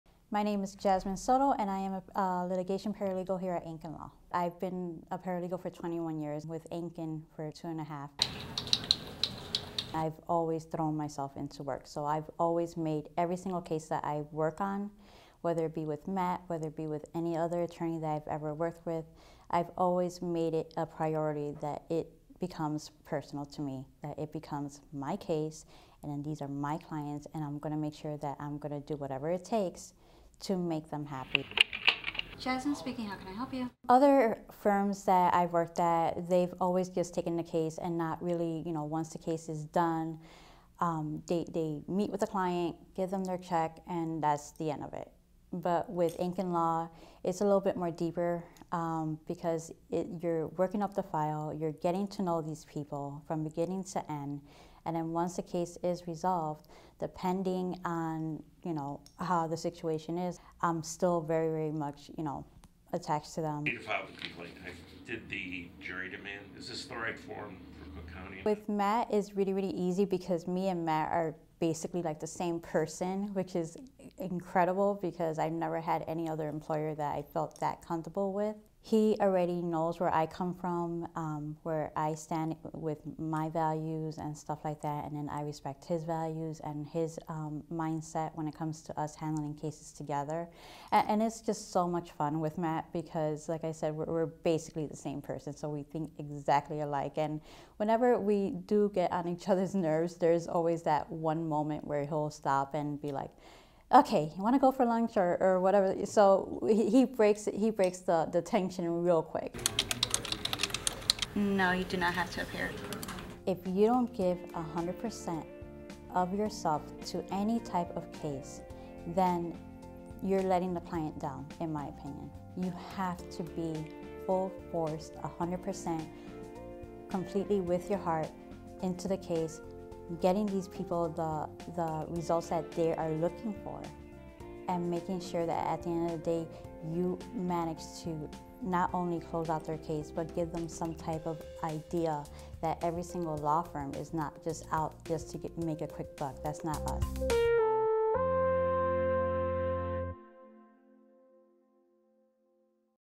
A legal assistant who makes EVERY case personal
Meet Ankin Law legal assistant Jasmine Soto and what motivates her to go above and beyond for our clients.